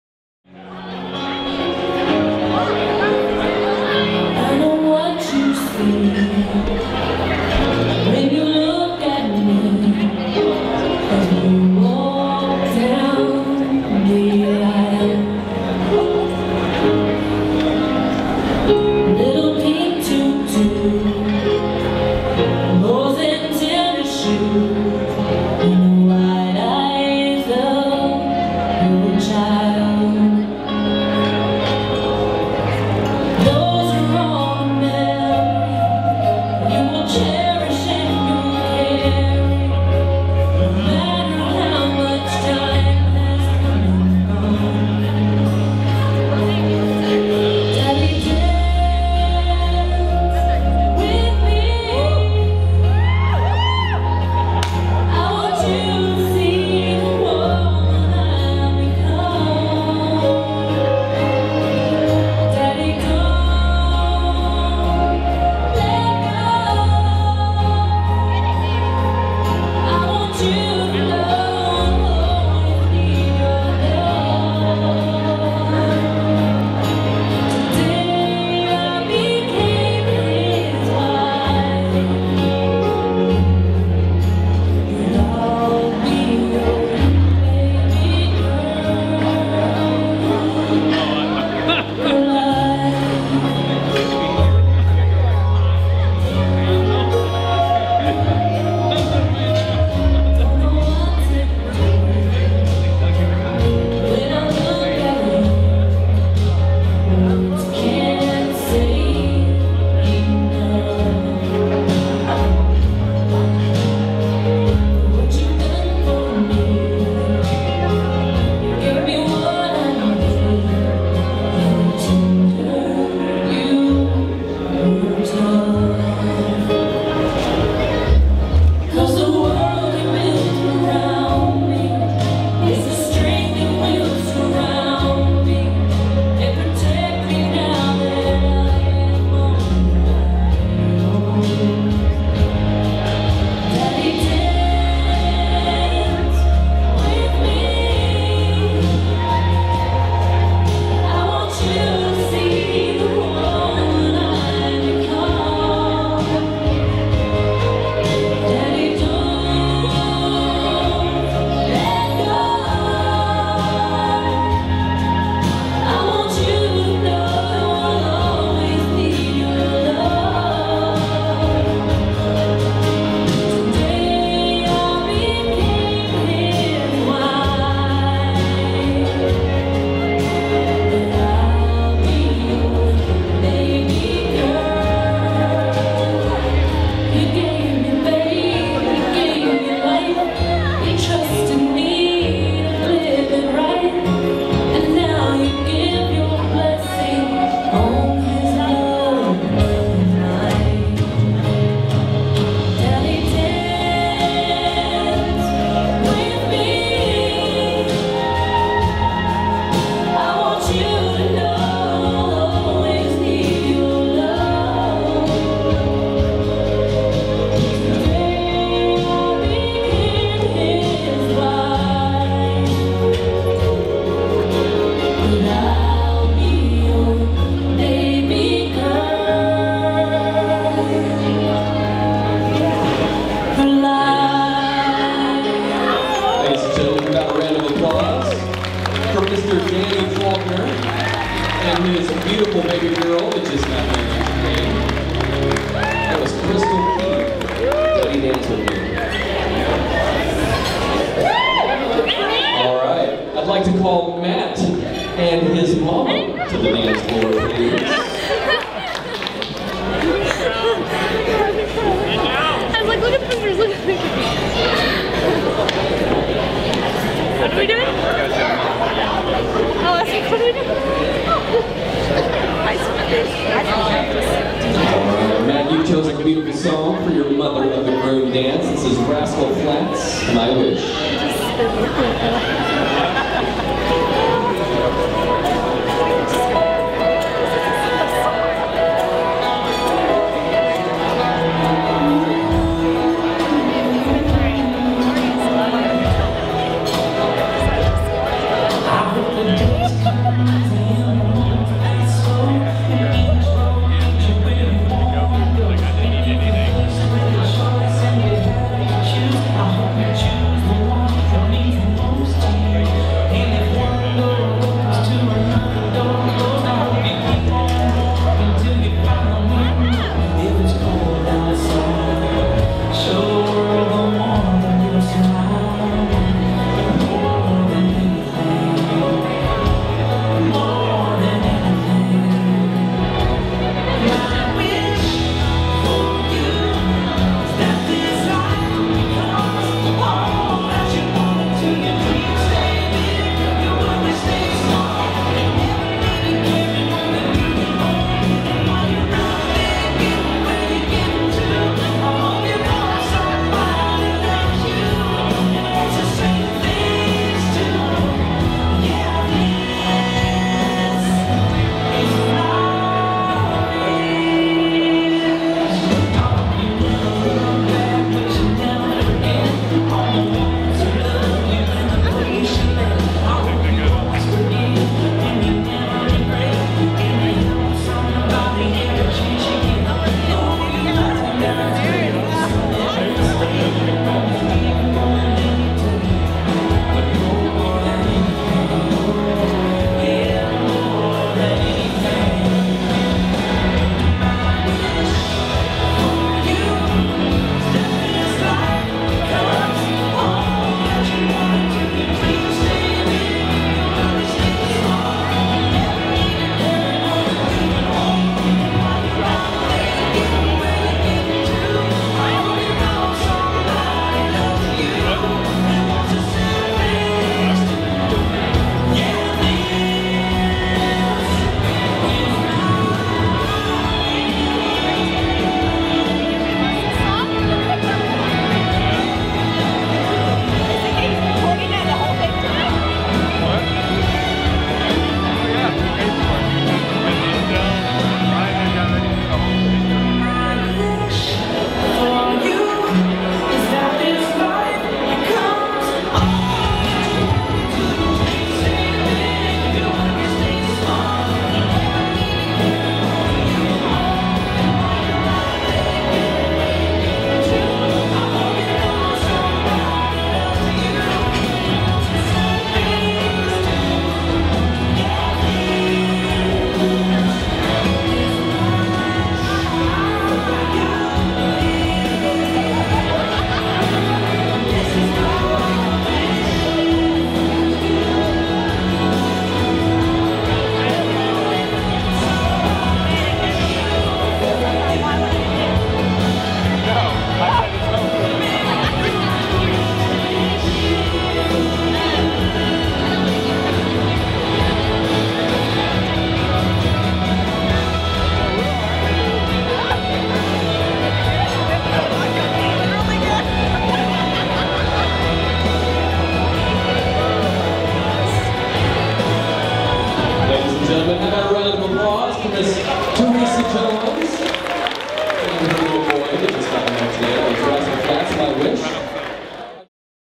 Danica and Matt's Wedding Video - Part 7 - Formal Dances
Danica's Wedding Video at the Chapel at Ana Villa in Dallas Texas by the videographers at DTX Weddings